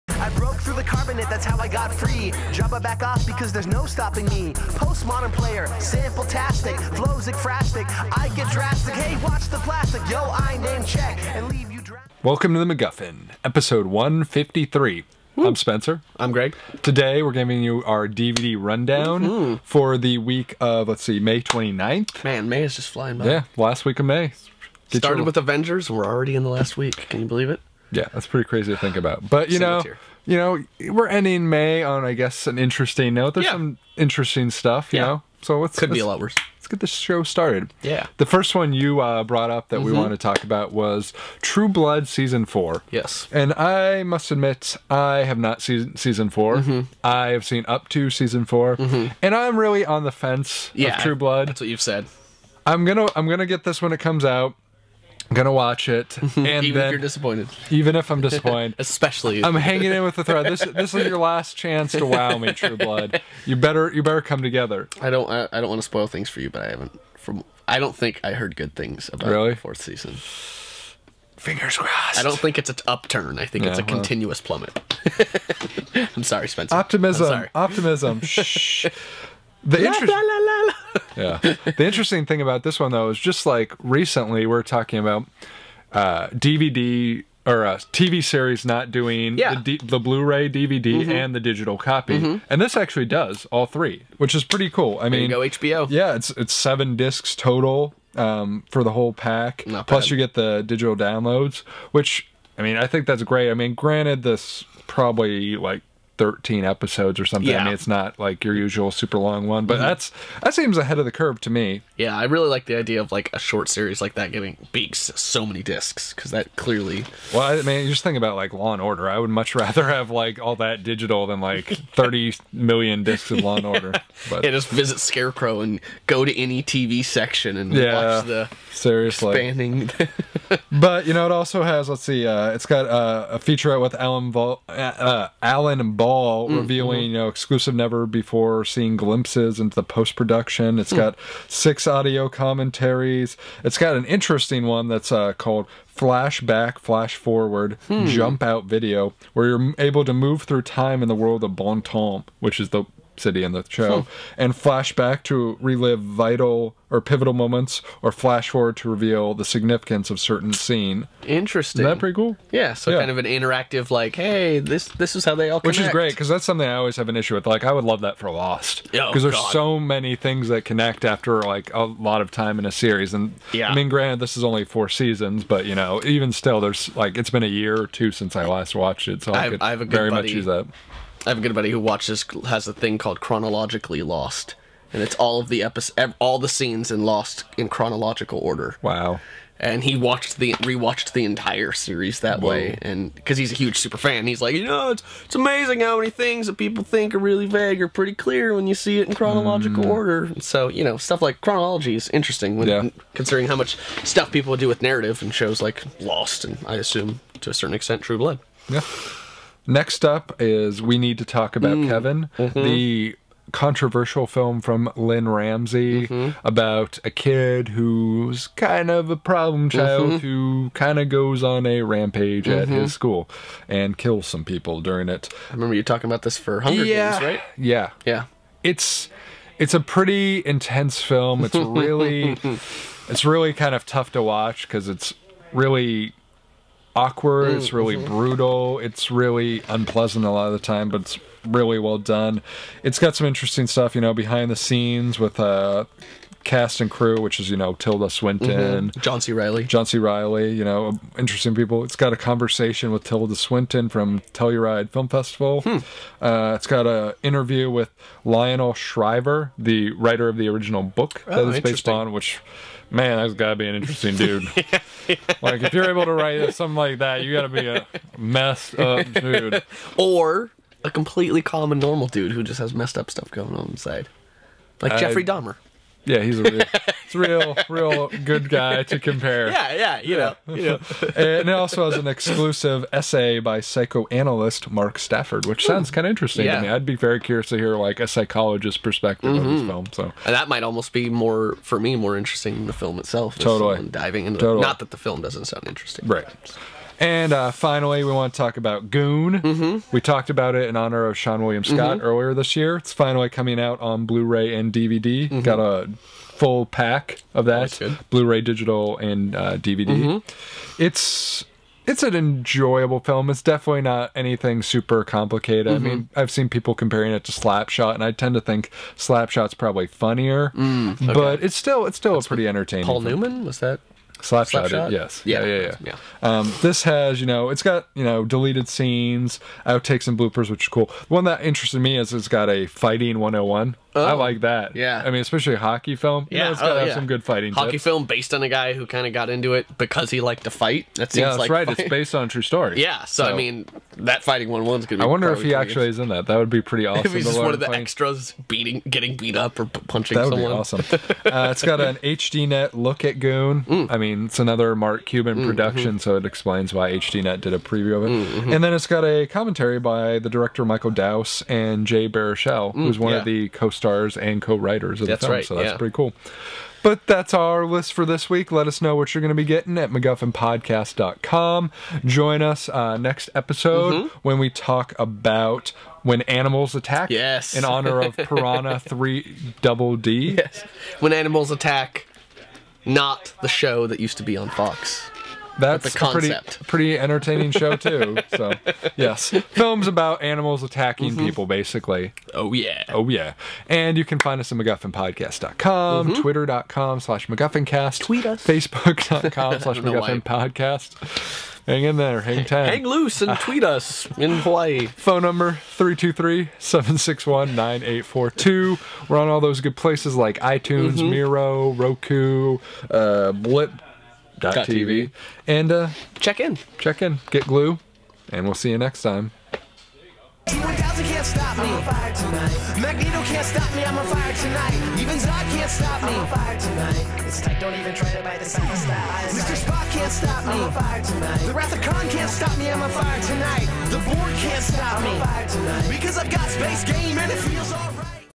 DVD Rundown 5-29-2012 - The MacGuffin
Spencer Fornaciari and Greg Thomas of The MacGuffin give their DVD rundown for May 29th, 2012. In this episode they discuss True Blood, We Need to Talk About Kevin, and Goon.